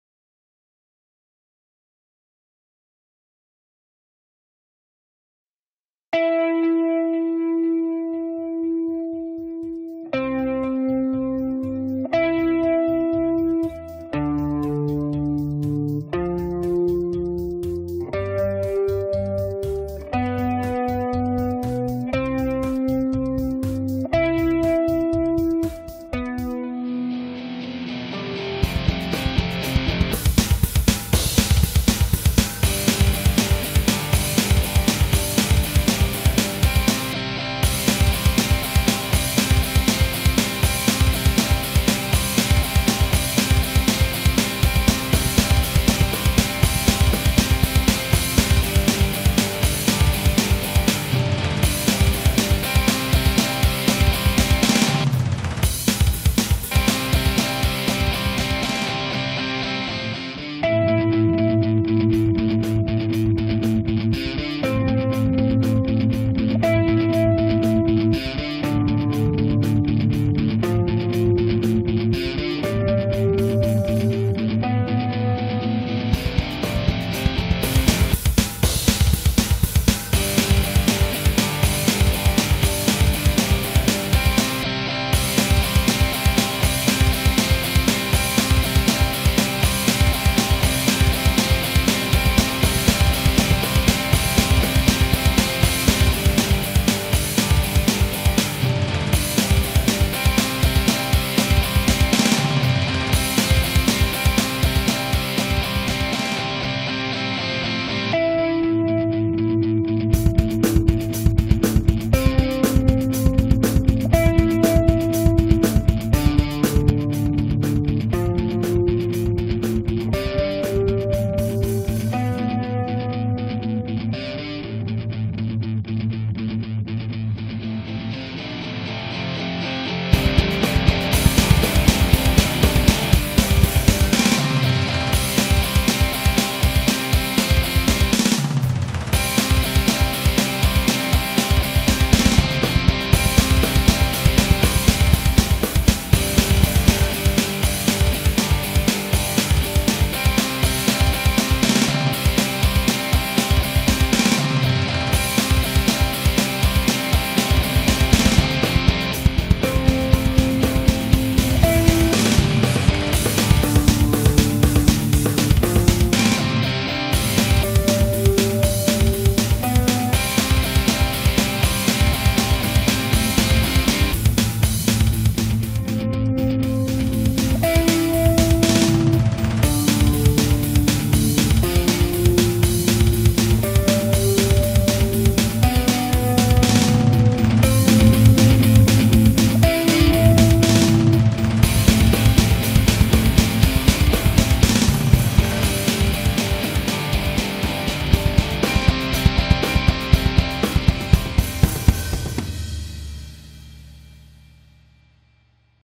für UDO 2
Hallo Udo , ich poste dir mal eine zweite Version!
So kannst du alles verfolgen was die Musik und und den Einsatz vom Text betrifft!
Ich habe sowas aber noch nie gemacht, einen Song nach einem Text zu erstellen, dafür bin ich warscheinlich zu blöde!
Habe da etwas wiederholt und der Texteinsatz sollte nach dem Tusch auf spur 5 und den darauf folgenden  Guitarreneinsatz erfolgen.
wiederholt sich 2 Mal! den Rest würde ich einfach Instrumental ohne
Gesang laufen lassen.
Ist nur ein Vorschlag von mir!
Du siehst die Spuren und ich hoffe dir damit einen kleinen Einblick zu vermitteln was man damit alles machen kann!
Ändere das aber gerne nach deinen Wünschen
wenn ich kann, so hast du die Möglichkeit mir Spur und Zeit zu nennen
um Änderungen vor zu nehmen.